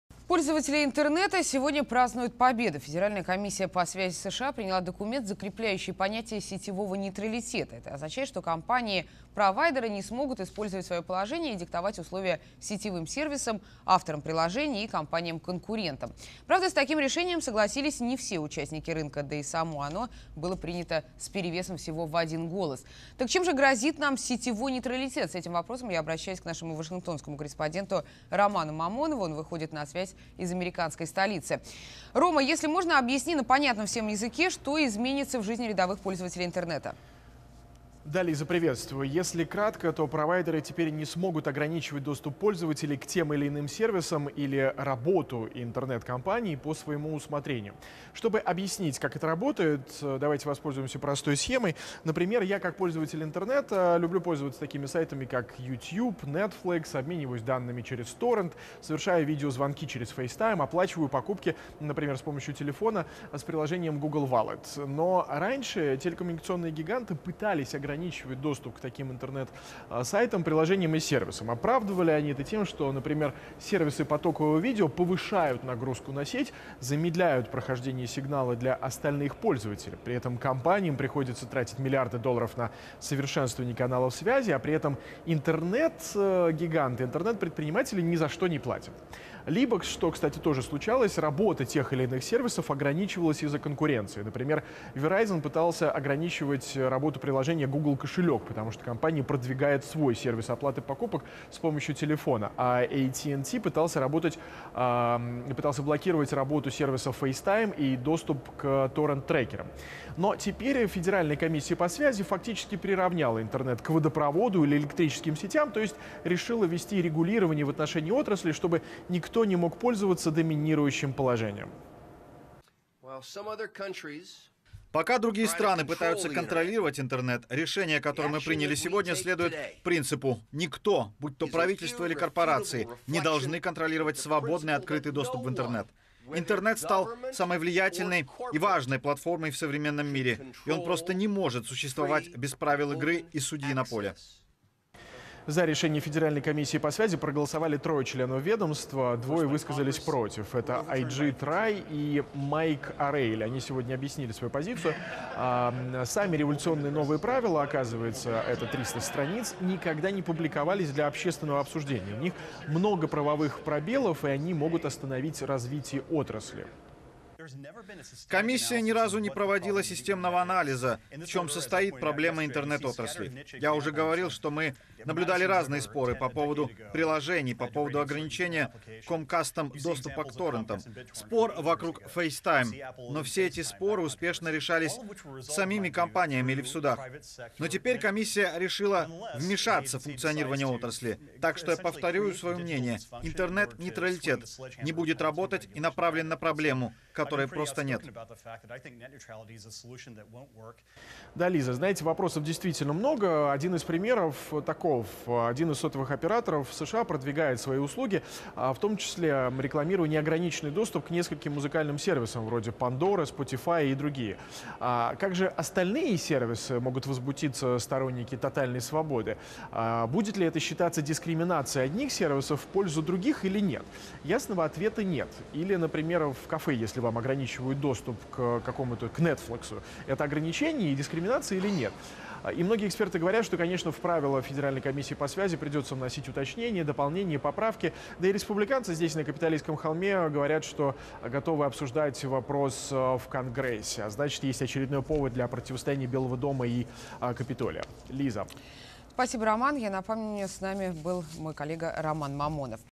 Игра по правилам: "сетевой нейтралитет" и свобода интернета
Пользователи интернета празднуют победу. Федеральная комиссия по связи США приняла документ, закрепляющий понятие "сетевого нейтралитета". Это означает, что компании-провайдеры не смогут использовать свое положение и диктовать условия сетевым сервисам, авторам приложений и компаниям конкурентам. Правда, с таким решением, согласились не все участники рынка. О том, что изменится для рядовых пользователей интернета рассказывает корреспондент RTVi Роман Мамонов.